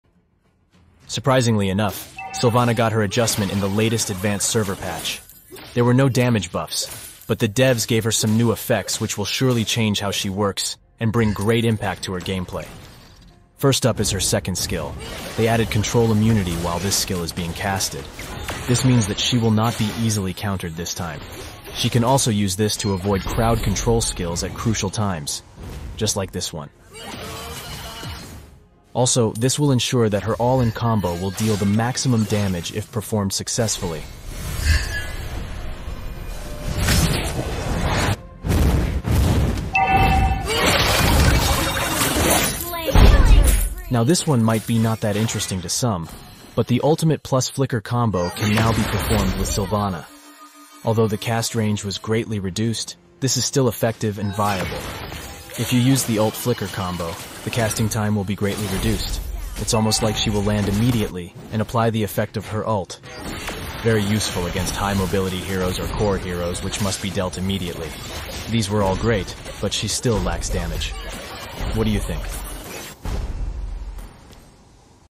NEW CC IMMUNE SILVANNA
Silvanna was also adjusted in the latest advanced server patch 1.7.72. This time, they made her 2nd skill immune to CC skills and she can now perform ult-flicker combo. What do you think of these changes?